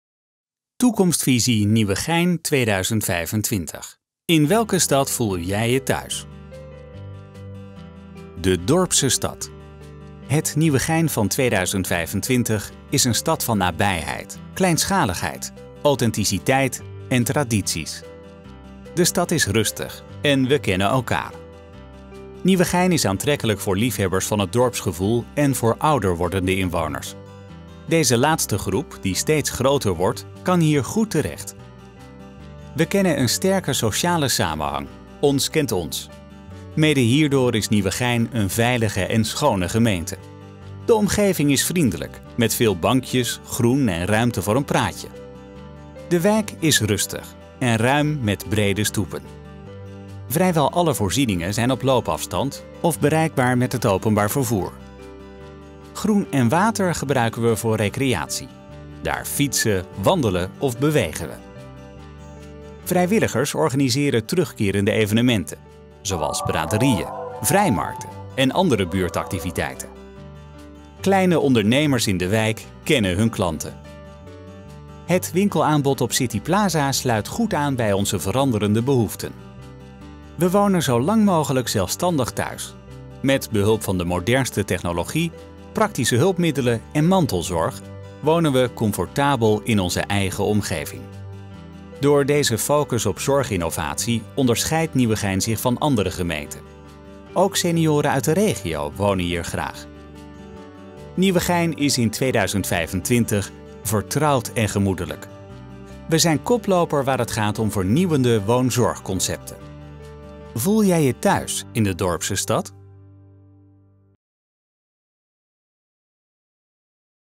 Dorpse stad
Het Nieuwegein van 2025 is een stad van nabijheid, kleinschaligheid, authenticiteit en tradities. De stad is rustig en we kennen elkaar.

Nieuwegein is aantrekkelijk voor liefhebbers van het dorpsgevoel en voor ouder wordende inwoners. Deze laatste groep, die steeds groter wordt, kan hier goed terecht. We kennen een sterke sociale samenhang : ‘ons kent ons’. Mede hierdoor is Nieuwegein een veilige en schone gemeente.

De omgeving is vriendelijk; met veel bankjes, groen en ruimte voor een praatje. De wijk is rustig en ruim met brede stoepen. Vrijwel alle voorzieningen zijn op loopafstand of bereikbaar met het openbaar vervoer.

Groen en water gebruiken we voor recreatie. Daar fietsen, wandelen of bewegen we.
Vrijwilligers organiseren terugkerende evenementen, zoals braderieën, vrijmarkten en andere buurtactiviteiten.

Kleine ondernemers in de wijk kennen hun klanten. Het winkelaanbod op City Plaza sluit goed aan bij onze veranderende behoeften.

We wonen zo lang mogelijk zelfstandig thuis. Met behulp van de modernste technologie, praktische hulpmiddelen en mantelzorg wonen we comfortabel in onze eigen omgeving.
Door deze focus op zorginnovatie onderscheid Nieuwegein zich van andere gemeenten. Ook senioren uit de regio wonen hier graag.

Nieuwegein is in 2025  vertrouwd en gemoedelijk.  We zijn koploper waar het gaat om vernieuwende woonzorgconcepten.

Voel jij je thuis in de dorpse stad?